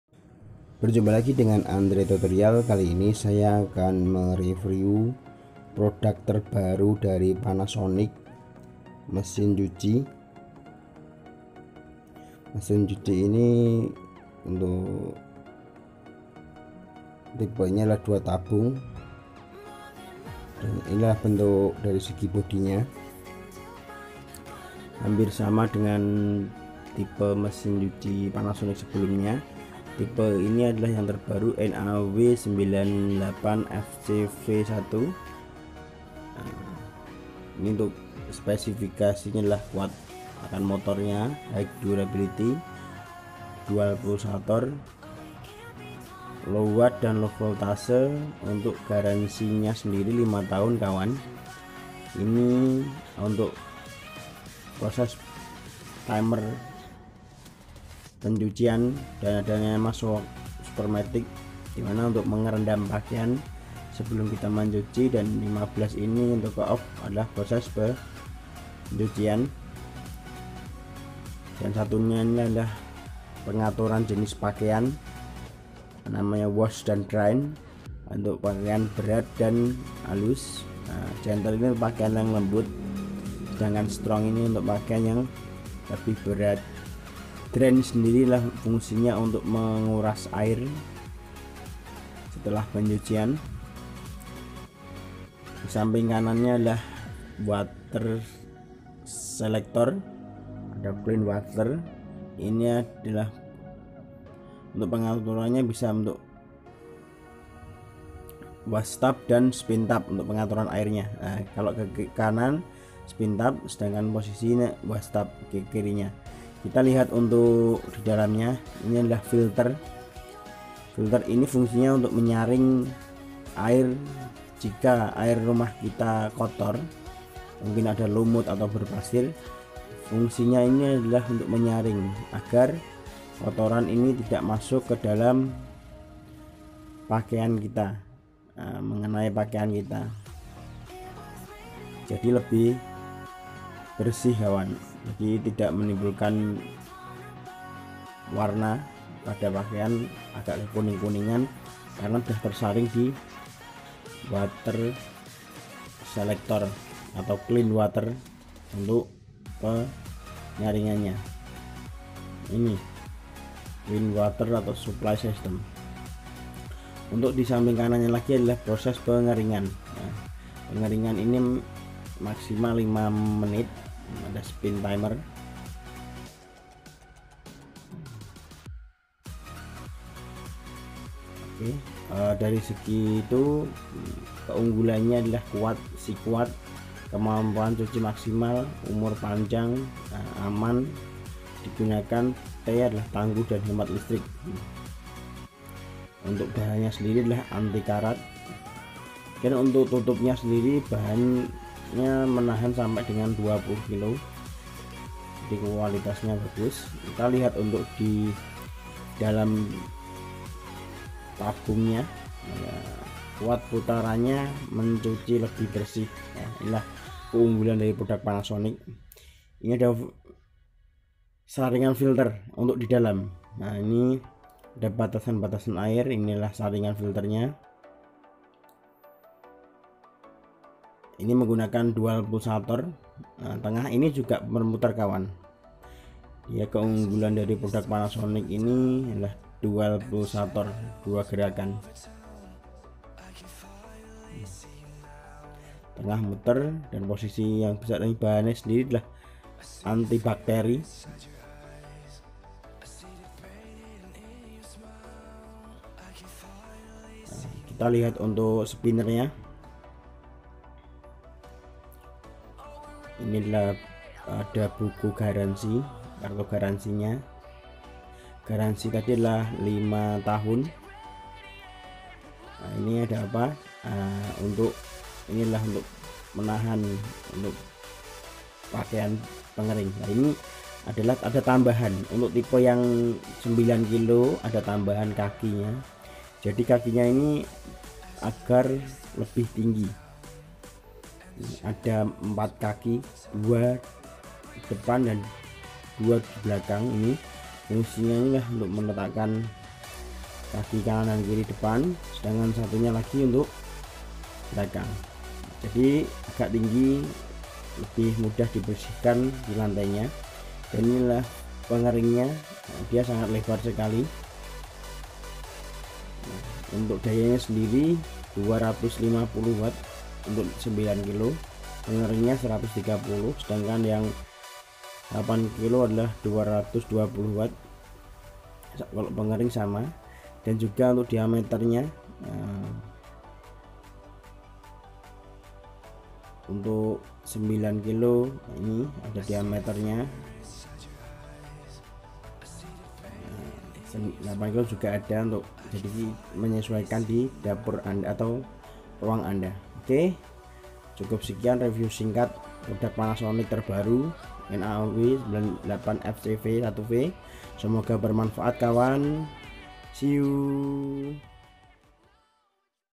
SI KUAT!!! Mesin cuci panasonic NA-W98FCV (Review)
Panasonic mengeluarkan mesij cuci type terbaru yaitu sikuat
Mesin cuci tangguh dan mumpuni
Mnjdi sahabat anda sekeluarga untuk membantu pekerjaan dirumah anda

Selamat membeli